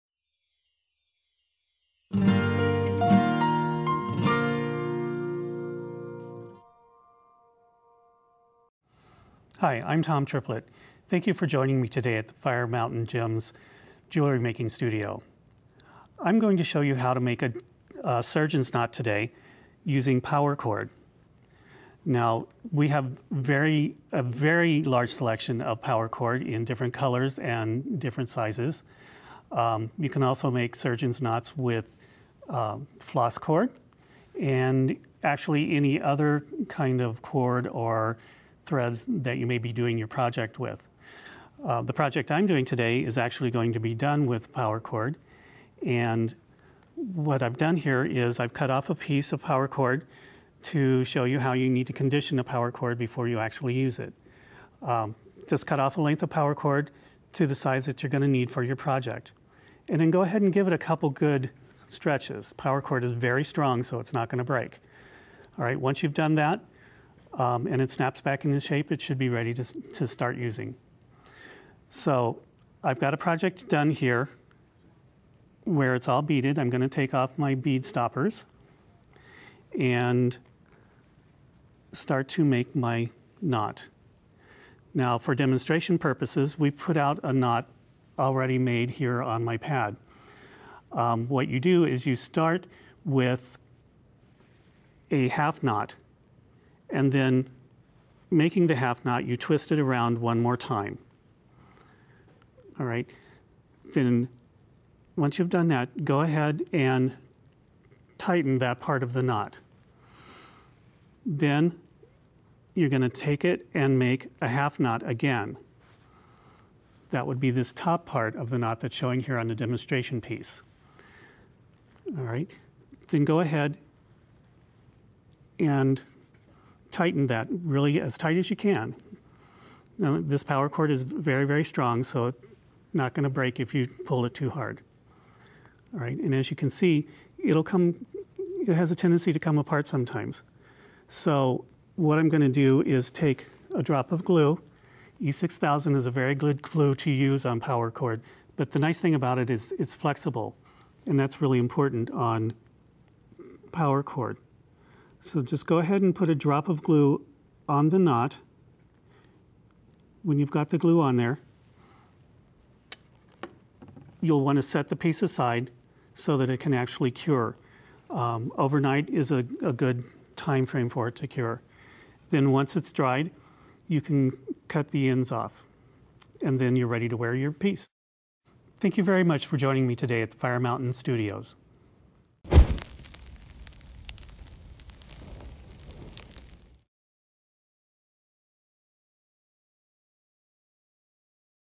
Finishing Elastic and Powercord®
Tom shows you how to finish elastic stretch cording (like Powercord), walking you through the process of creating a surgeon's knot. Find this, and hundreds of other jewelry-making how-to videos, at Fire Mountain Gems and Beads.

🔴Want a FREE catalog?

Socialize with us here!